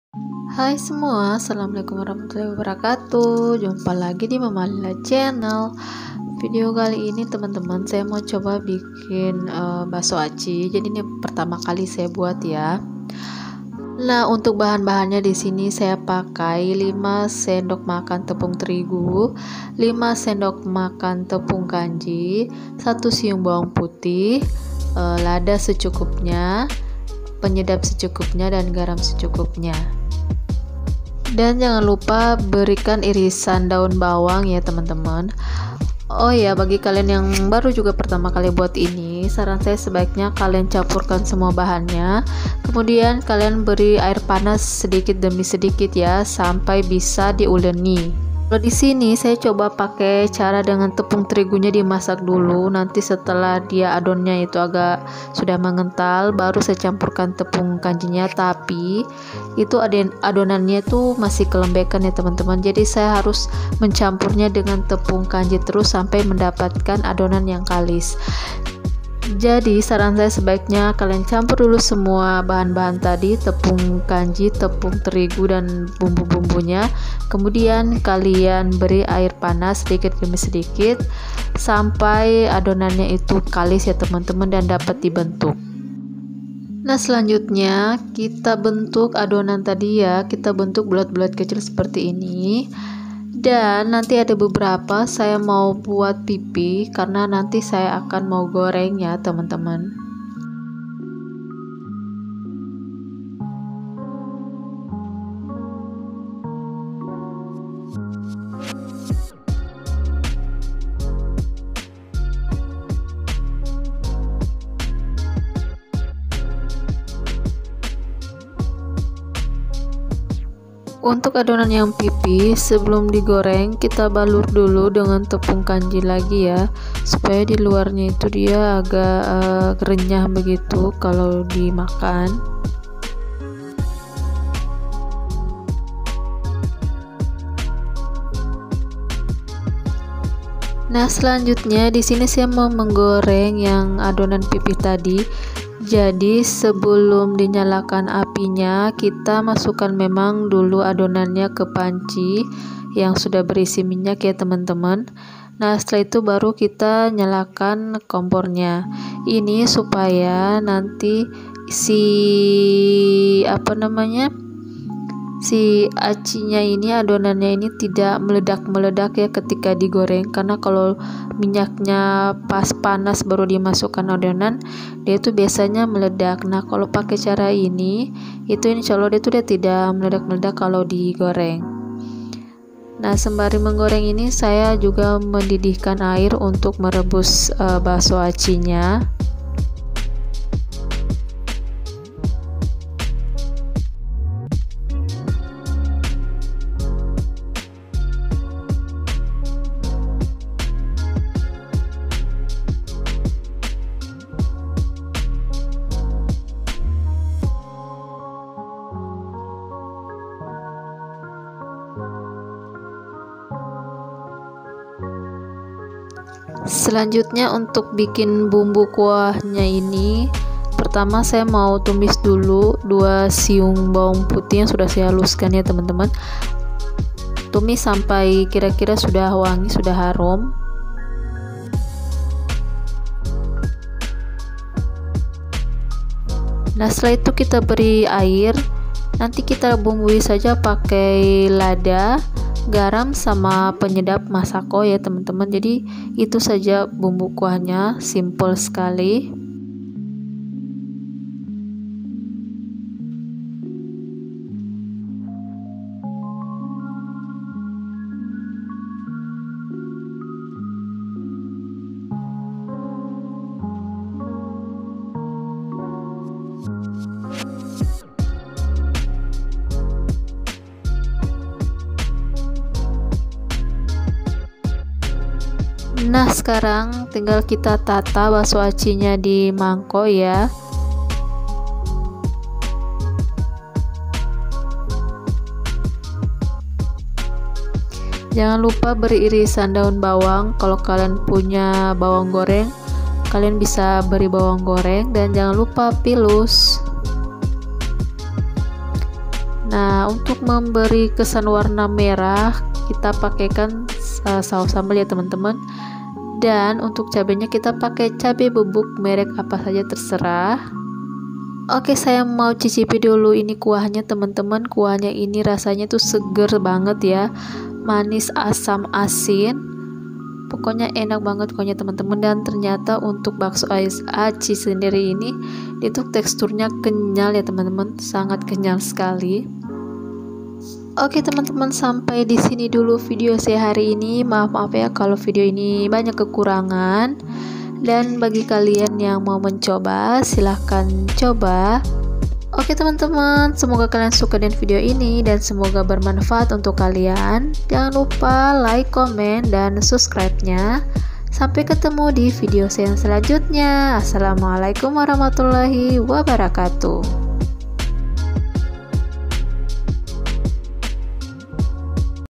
Lakukan cara ini ketika pertama kali membuat baso aci || cara membuat baso aci kuah pedas
Jadi ini pertama kali sy membuat dan makan baso aci. 2kali percobaan sy gagal temen2 krn proses yg saya lakukan adalah memasak tepung terigunya dahulu, kemudian setelah itu dicampur dgn tepung tapiako. Tp cr ini membuat adonan kelembekan dan susah untuk kalis. Jadi saran saya adalah kalian campur semua bahan menjadi satu dan berikan air panas sedikit demi sedikit sampai adonan jadi kalis..

Bahan baso aci
5 sdm tepung terigu
5 sdm tepung tapioka/kanji
1 siung bwng putih haluskan
Lada
Garam
Penyedap
Daun bawanf

Bumbu kuah
2 siun6g bawang putih haluskan dan tumis sbntr
Lada
Garam
Penyedap
Saos sambel
Cabe bubuk
Bawang goreng (opsional)
Daun bawang
Pilus